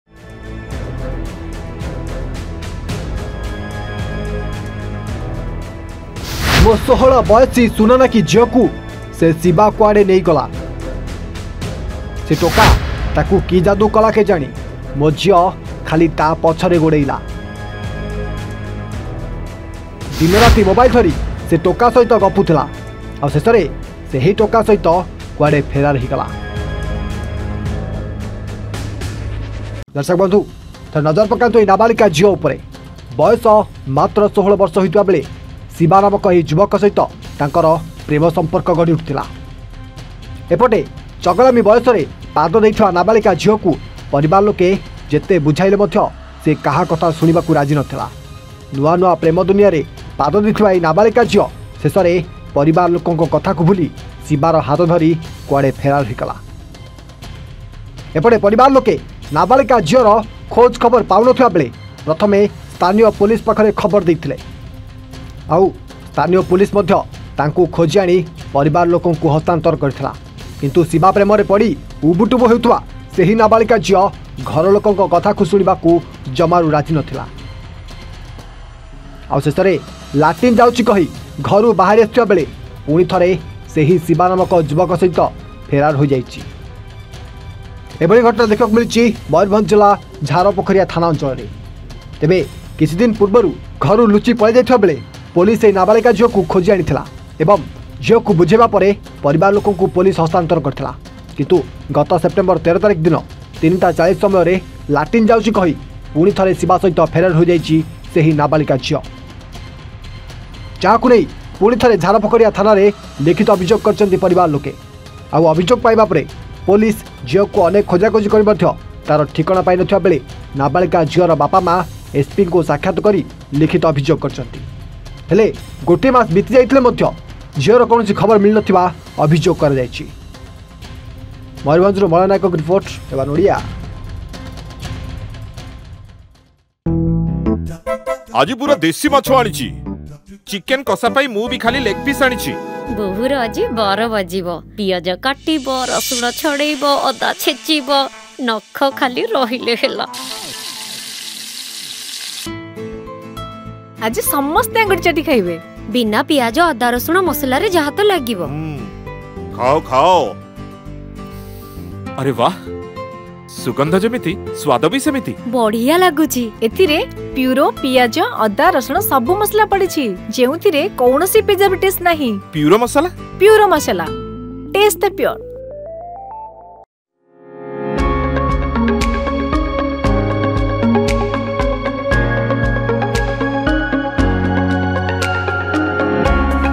ଲାଟିନ ଯିବ ବୋଲି କହି ବାରିପଛରେ ପ୍ରେମିକ ସହ ଫେରାର ହେଲା ଅନୁ | Love case | Cuple
🔆MORE UDATE🔆